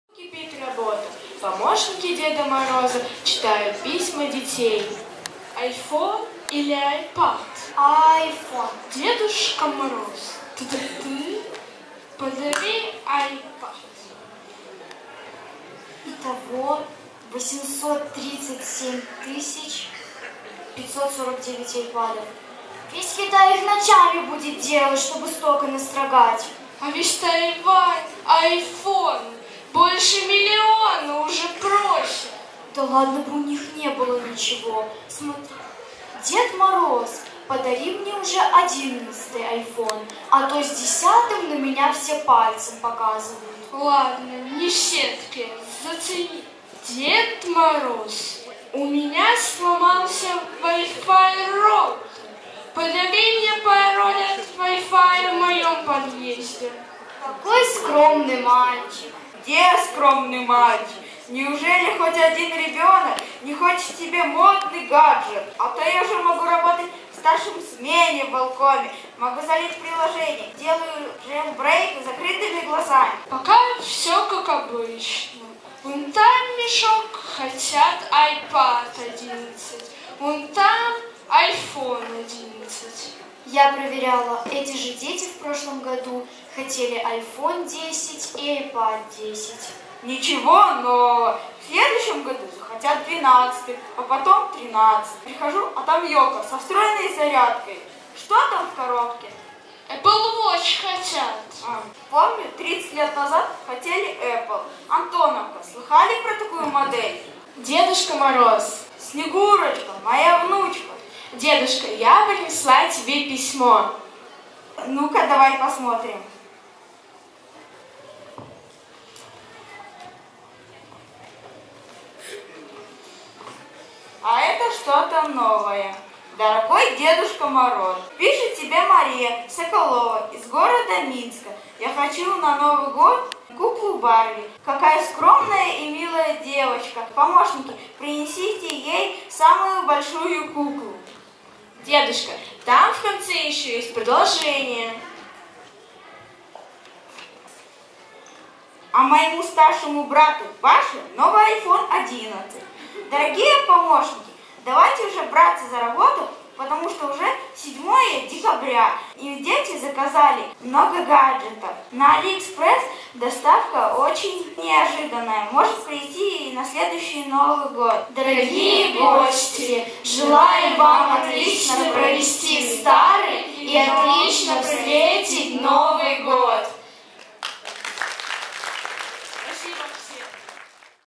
СШ №111 г. Минск Театральная постановка "Дед Мороз не верит в детей"
6 "Г" класс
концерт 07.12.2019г